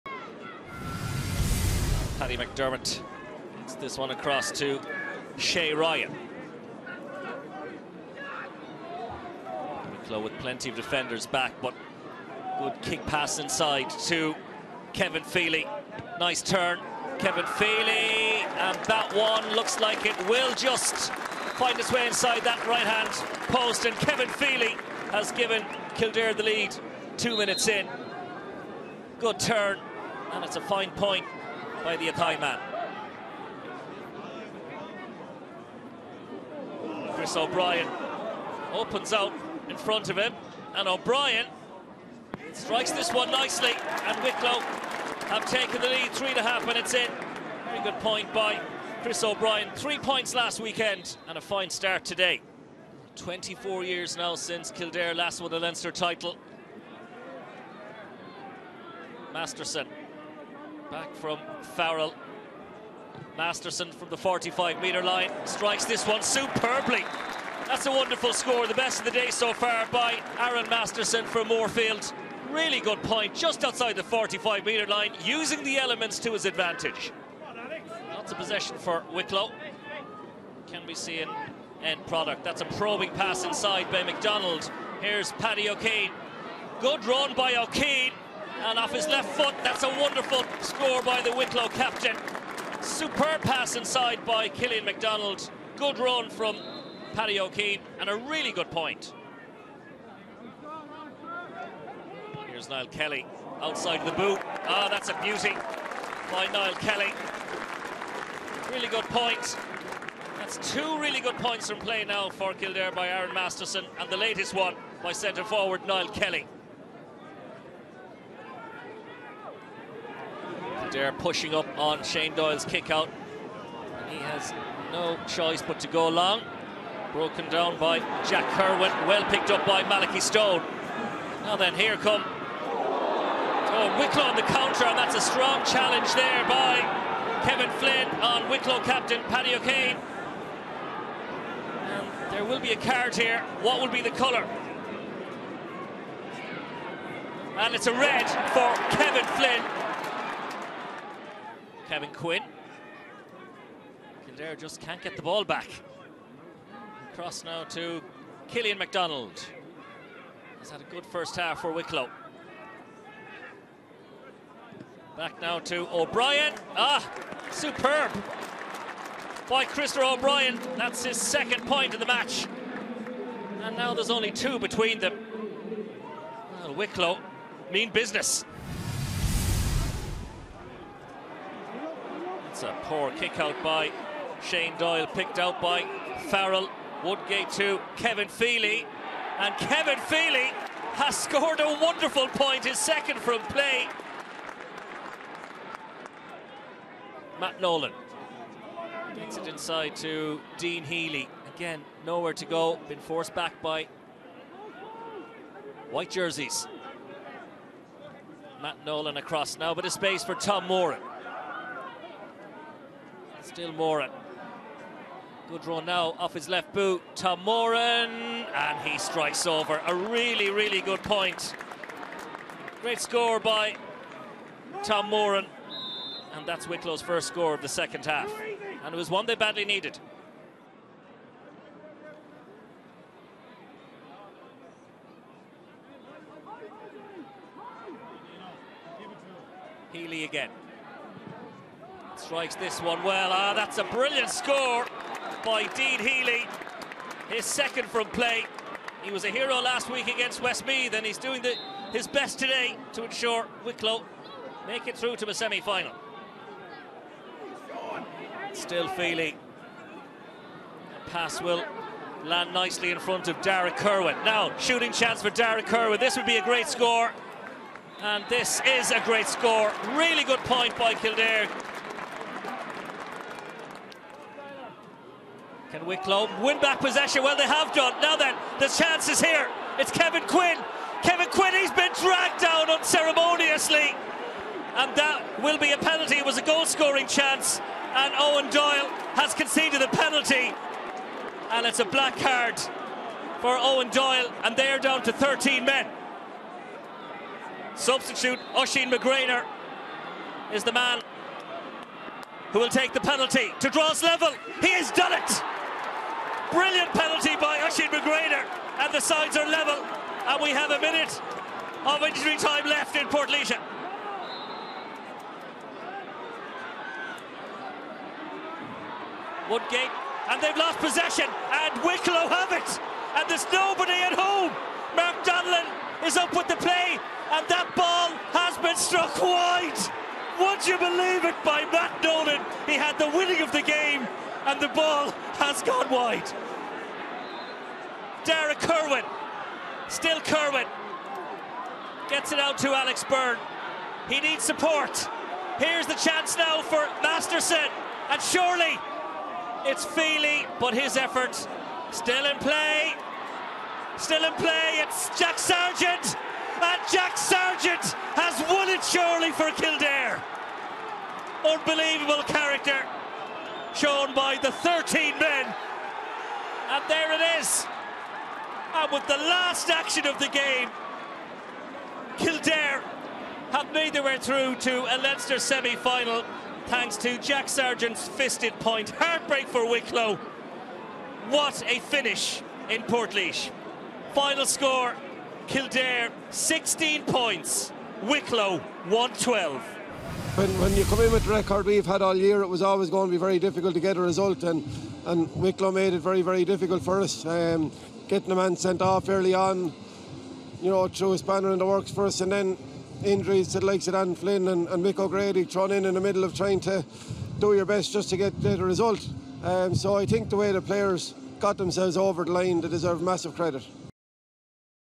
Kildare win it at the death against Wicklow | Wicklow 1-12 Kildare 0-16 | Leinster SFC highlights
Jack Sargent fisted over a 75th-minute winning point as Kildare booked a place in a Leinster SFC semi-final, but only after a crazy injury time period when they almost let the game slip from their grasp.

Glenn Ryan's team, who lost all seven games in the league and were relegated from Division 2 of the Allianz Football League this year, were three points up and looking comfortable half way through the four additional minutes despite the 30th-minute sending-off of Kevin Flynn.

They were in controlled possession of the ball in the middle of the field but Kevin Quinn nipped in to dispossess Eoin Doyle and race towards goal. Doyle raced back but the only way he could stop Quinn was a rugby tackle outside the square. The referee awarded a penalty and showed a black card to Doyle.

Oisin McGraynor tucked away the penalty to level the scores - but there was more drama to follow.

Remarkably, Kildare coughed up possession again in the middle of the field, this time with goalkeeper Mark Donnellan miles out of the position. Wing-back Matt Nolan brought the ball forward but with the goal gaping his shot from the 45 bounced agonisingly wide of the post.

Much to the annoyance of Wicklow manager Oisin McConville, Kildare were allowed to continue play into a fifth minute of injury-time and from there they worked a winning score. Kevin Feely’s wayward shot was kept in play by at the back post by Niall Kelly who worked it back to Sargent to put over the bar to the relief and delight of Kildare.